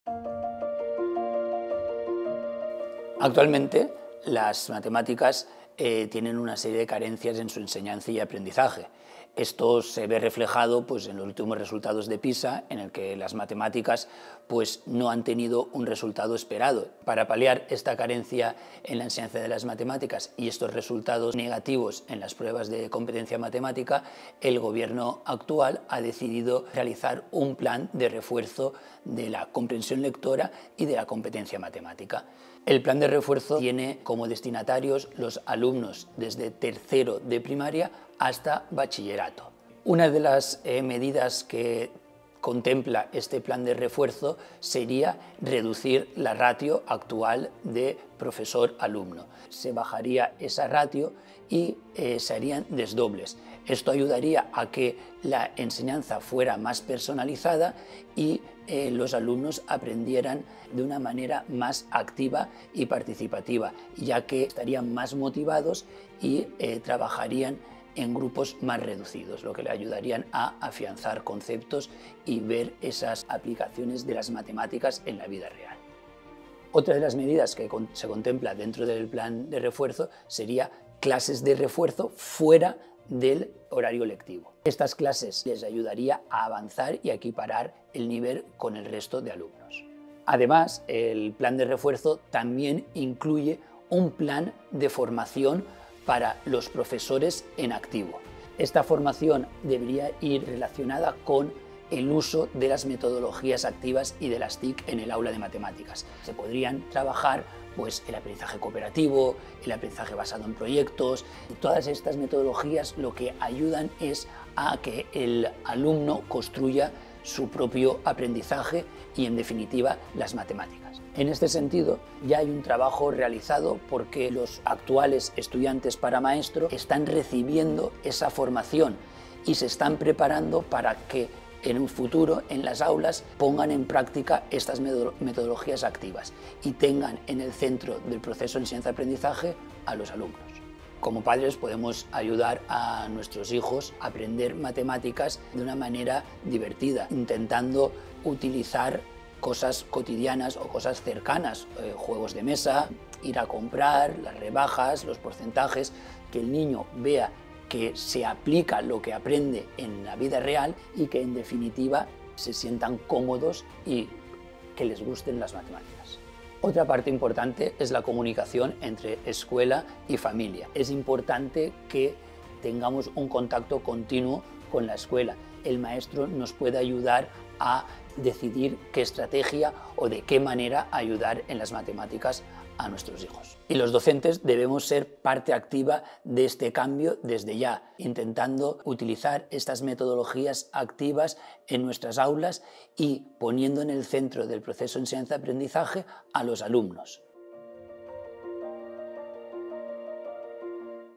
Descifrando la Realidad | NUEVO PLAN DE MEJORA EN MATEMÁTICAS
Alberto Zapatera, profesor de magisterio de la universidad CEU Cardenal Herrera, aborda el nuevo plan de mejora en matemáticas y comprensión lectora que promueve el gobierno.